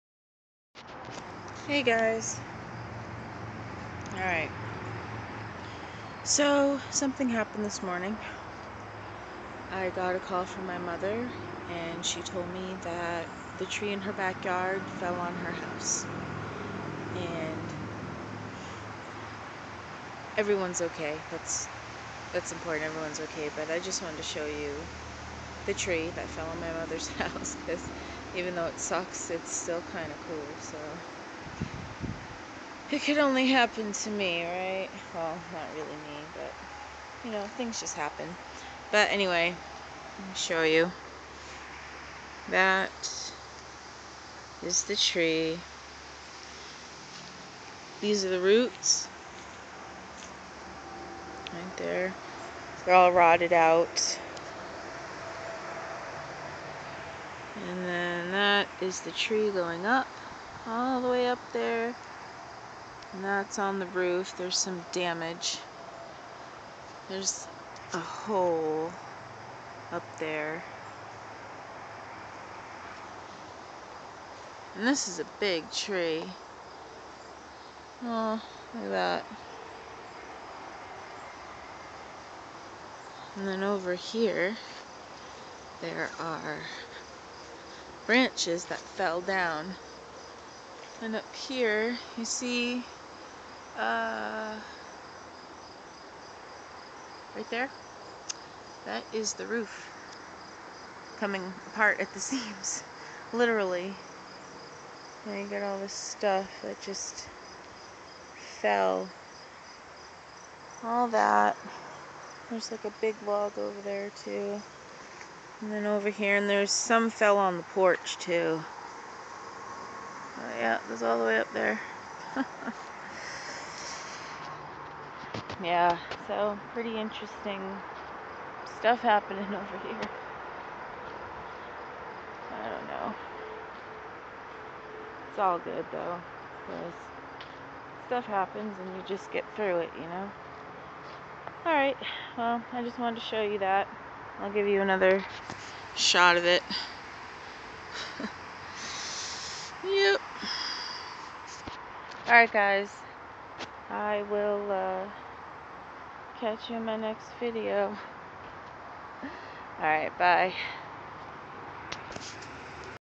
Shit Happens...
A tree fell on my mothers' house :(